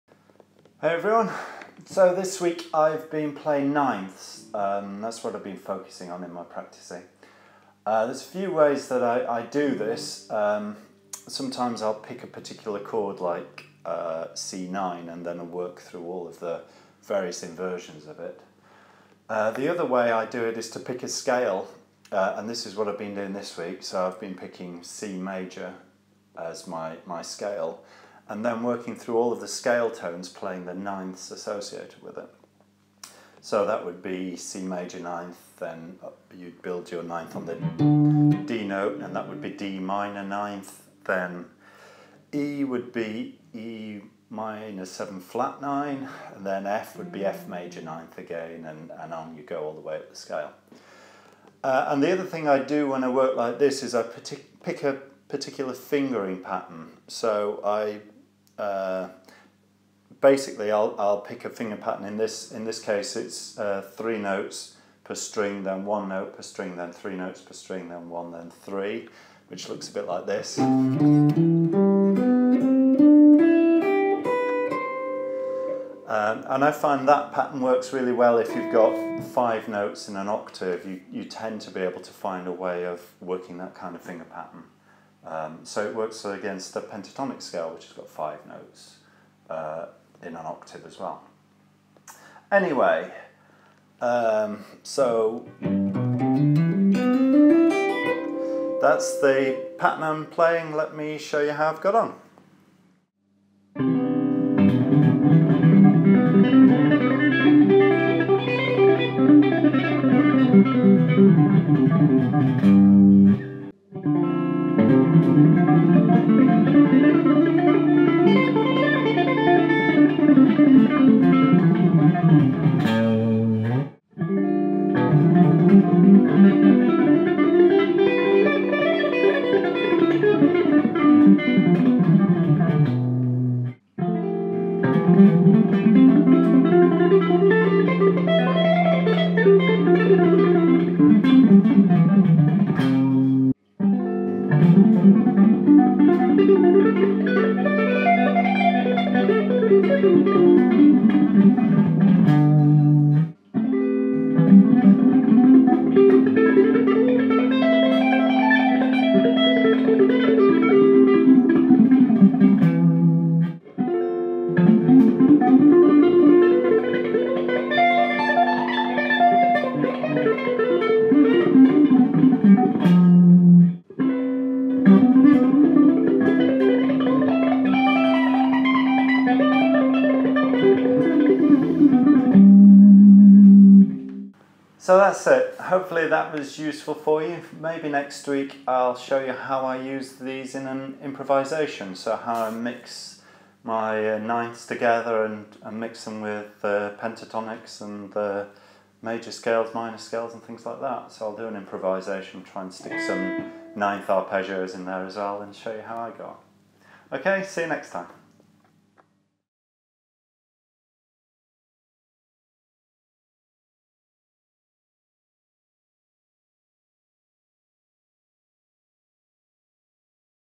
Playing 9th Arpeggios: Guitar Lesson
This week I've been playing Diatonic 9th Arpeggios up and down the guitar neck.

These are not the easiest fingering patterns - there are some big stretches! But see how you get on with them.

I've included the fingering charts to guide you...

1:45 Cmaj9
1:55 Dmin9
2:06 Em7b9
2:16 Fmaj9
2:25 G9
2:35 Amin9
2:46 Bm7b5b9
3:01 Cmaj9 (octave higher).

Guitar Tips, Tricks, Lessons. New Content Every Week!